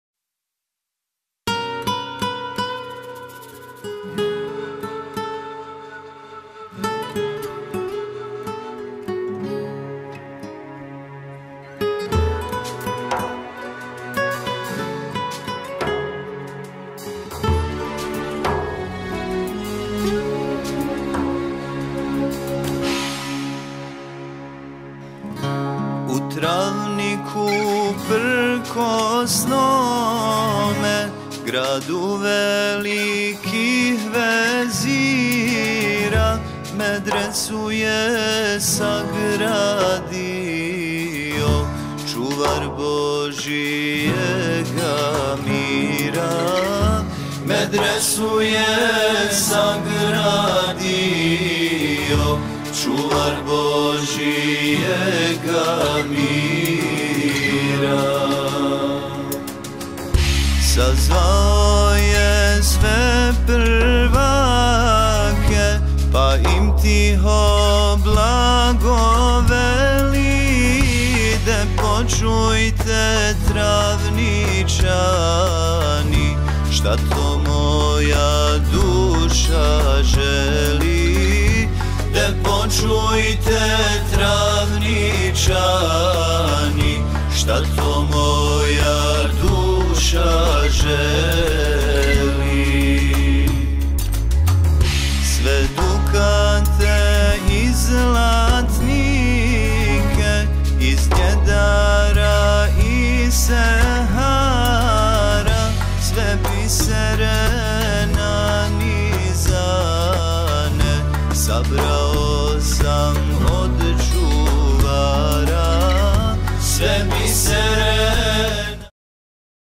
Medresa Travnik - teren za mali nogomet sa umjetnom travom
Elči Ibrahim-pašina medresa u Travniku, Teren za mali nogomet sa umjetnom travom u naselju Goleš kod Travnika.

Zahvaljujemo našem prijatelju Mehi Šiljku na ustupljenom snimku!